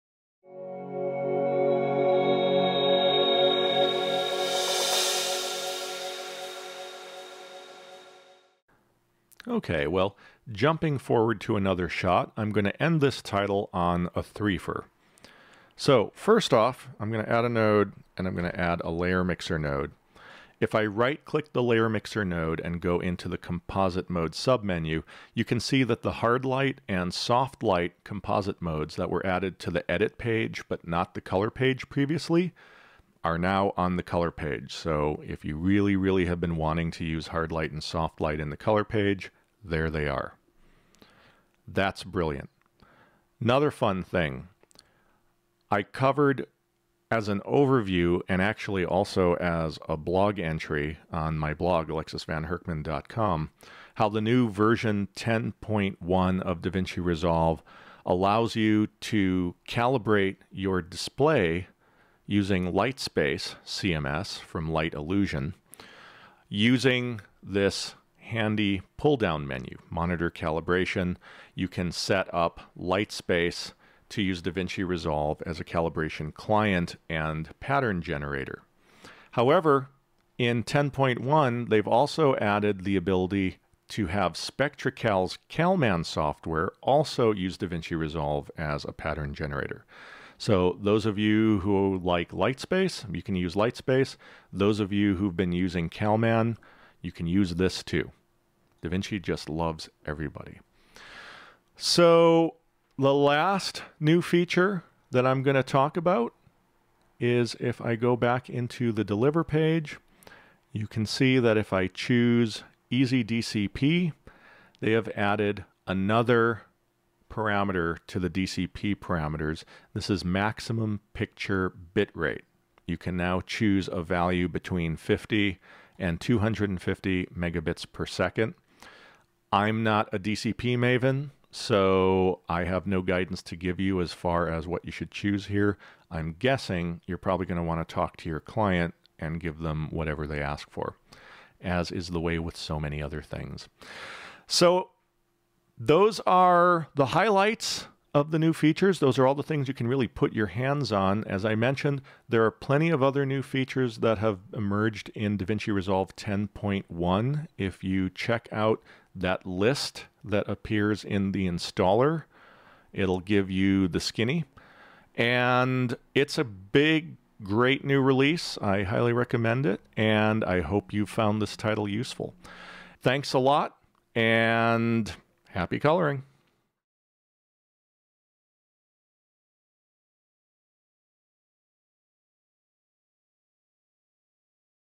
Lesson 7 Wrap up & Other Features in Resolve 10.1
In this video Alexis Van Hurkman talks about Using New Composite Modes, SpectraCal CalMAN and Resolve for Monitor Calibration, and Changing easyDCP Maximum Picture Bitrate in DaVinci Resolve 10.1